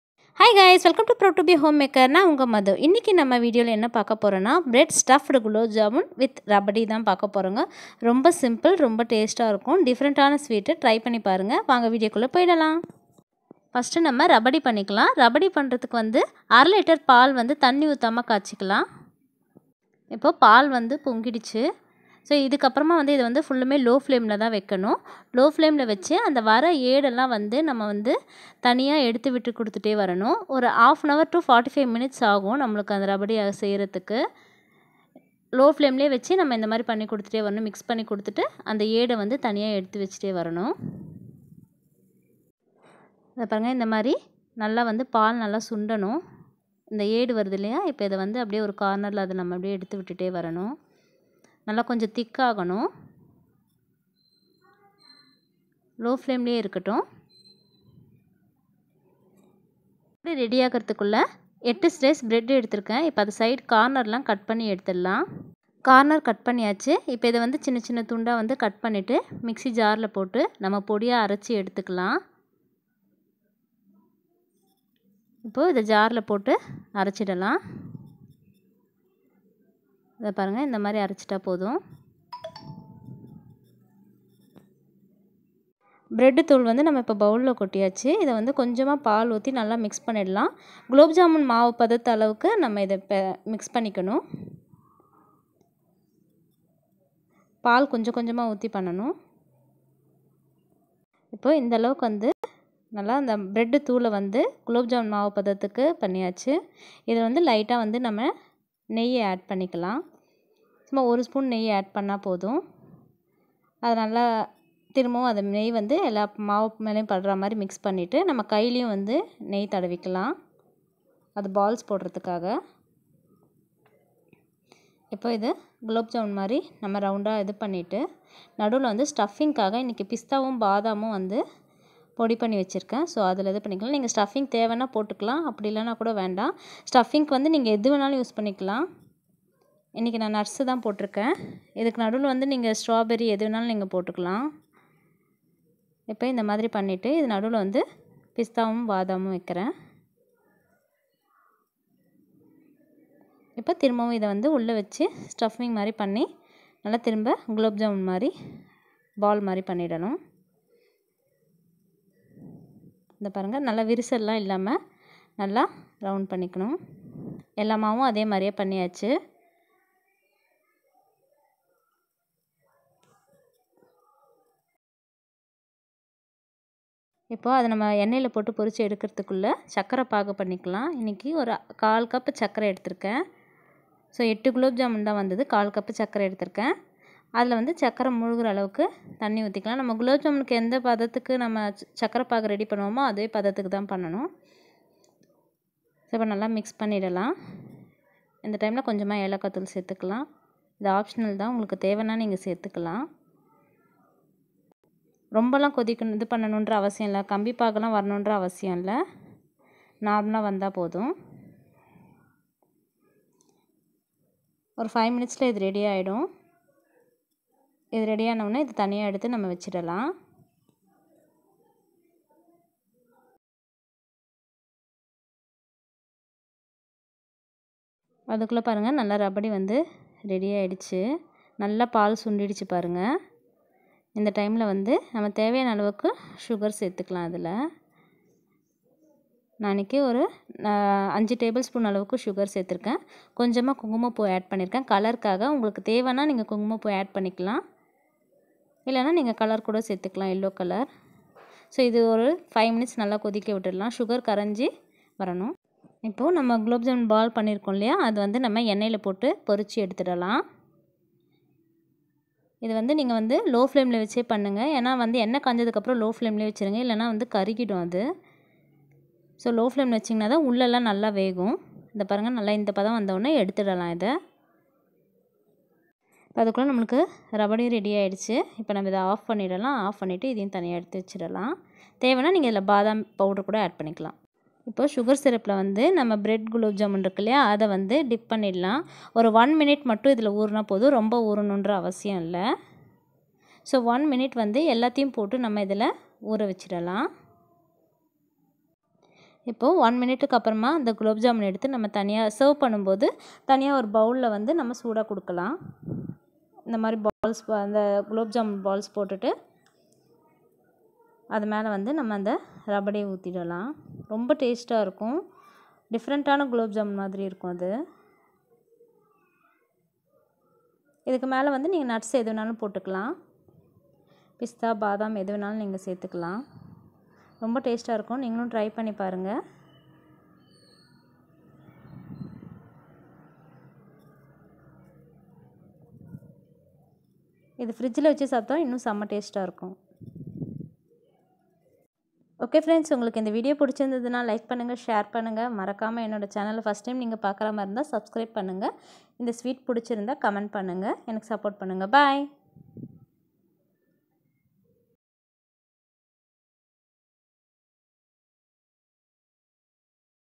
Bread stuffed gulab jamun with rabdi 😋😋 || Cook with comali kani recipe
milk
sugar
bread
nuts